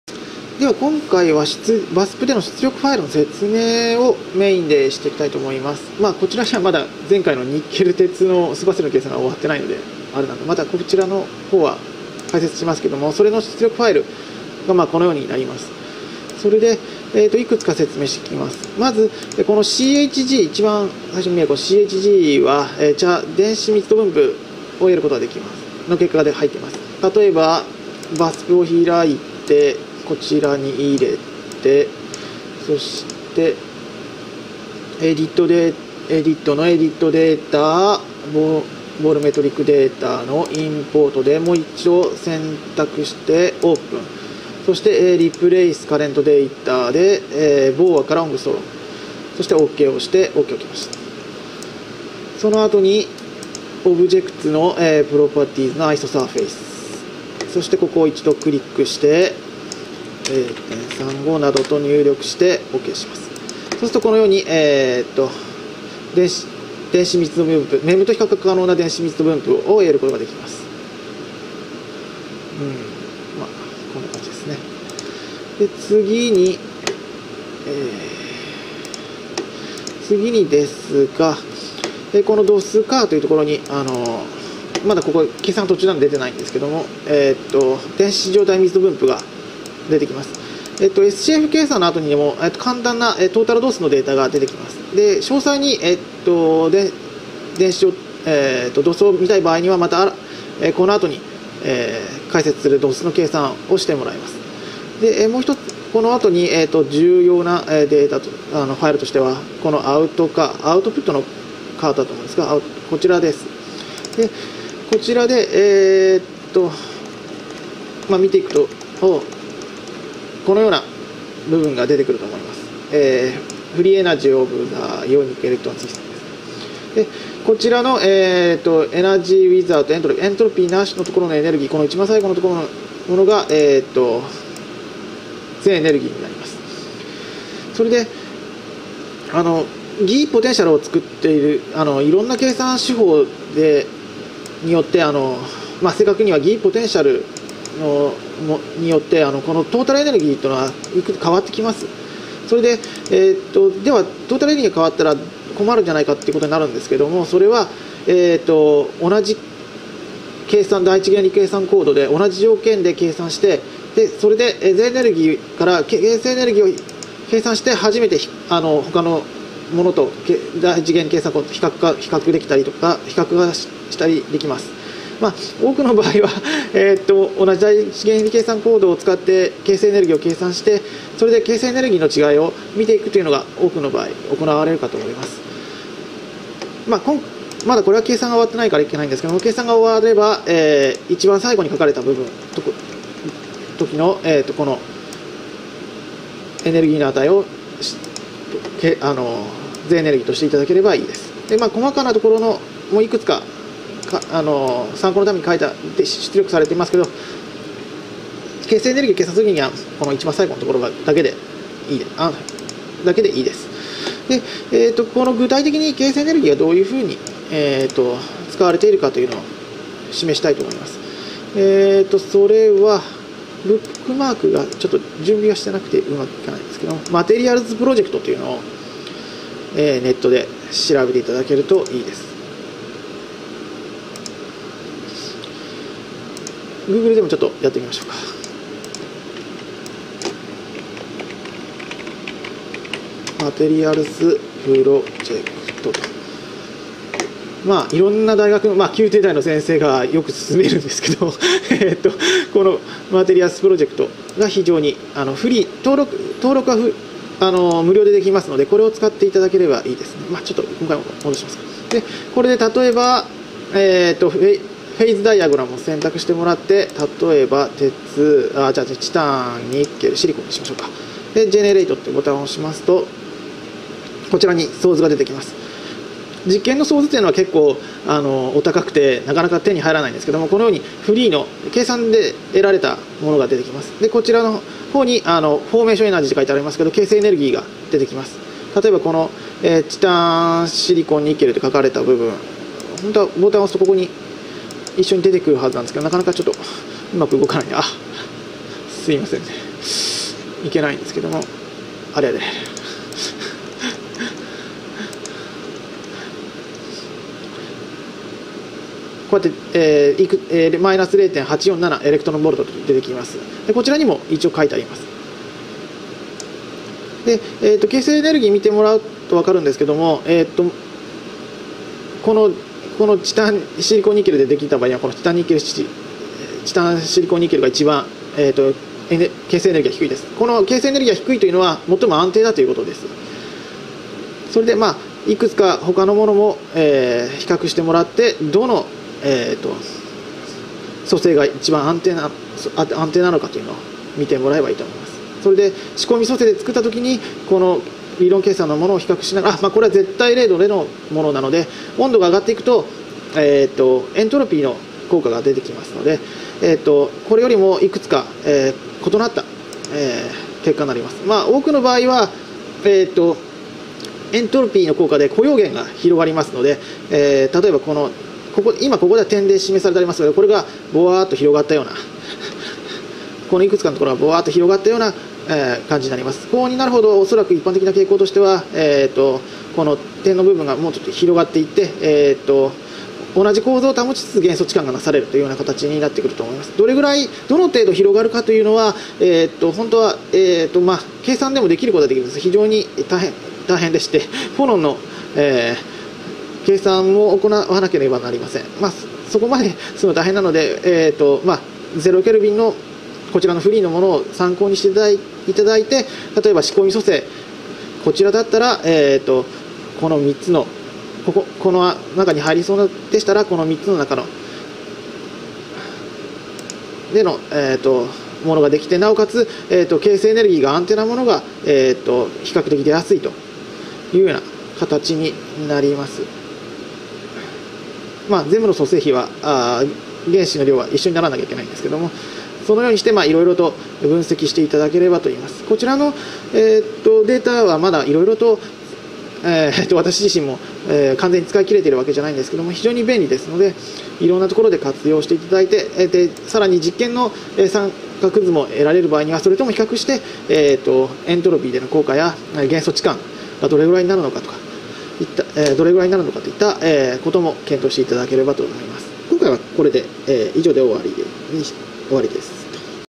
VASPの出力ファイルの説明
注意：CHGではなくCHGCARを用いてください。（説明が誤っていてすみません）
相図の固溶限については私の経験で正確なものではないので、相図については後ほどもう少し詳細な説明をします。